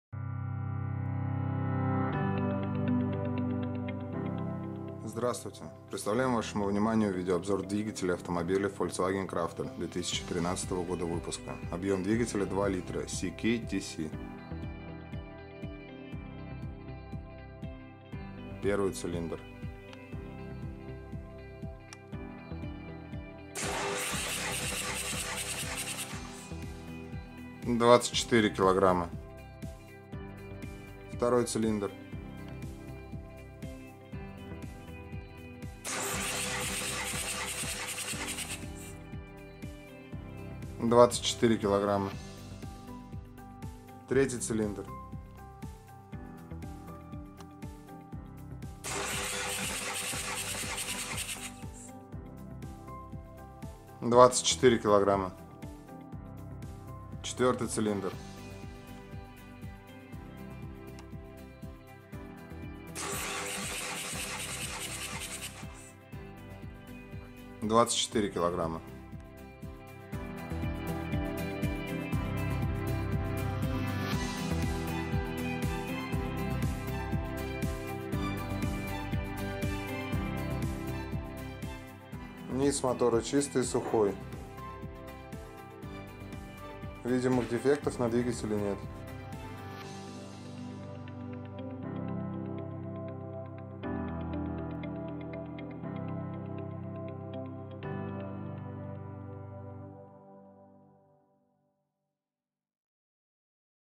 Двигатель VW для Crafter 2006-2016
Описание двигателя Двигатель VW для Crafter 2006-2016
2.0Л. 16V ДИЗЕЛЬ ТУРБО 2013Г. КОМПРЕССИЯ: 1Ц:24 2Ц:24 3Ц:24 4Ц:24 ХОЛОДНАЯ КОМПРЕССИЯ ЕСТЬ ВИДЕООБЗОР ДВИГАТЕЛЯ И ЗАМЕРА КОМПРЕССИИ КОНТРАКТНЫЙ ЕСТЬ СОПРОВОДИТЕЛЬНЫЙ ПАКЕТ ДОКУМЕНТОВ ГТД CKTC 03L100090S